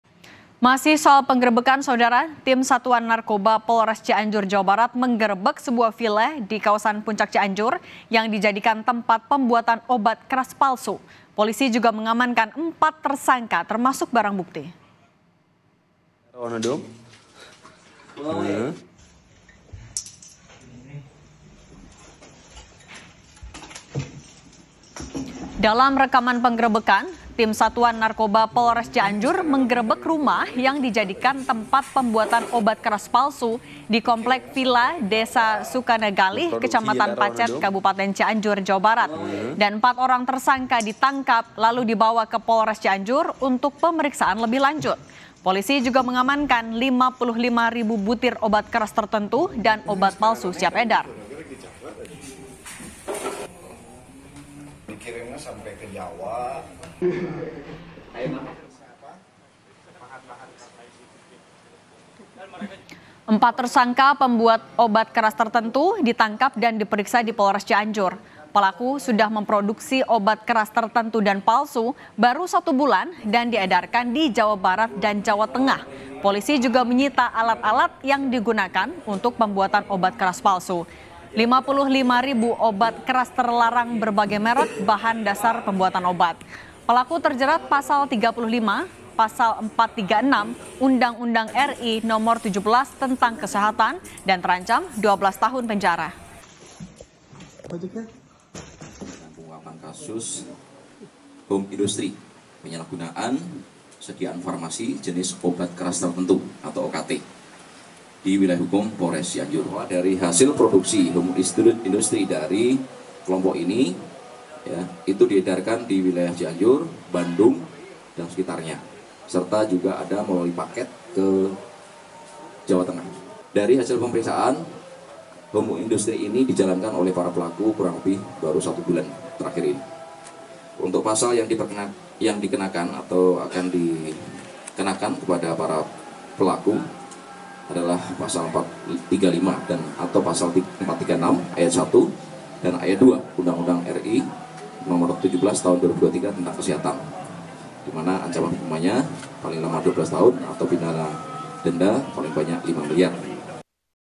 Polres Cianjur Gerebek Pabrik Obat Keras Palsu, Empat Orang Ditangkap
KOMPAS.TV - Polres Cianjur Gerebek Pabrik Obat Keras Palsu, Empat Orang Ditangkap

Kompas TV
Independen | Terpercaya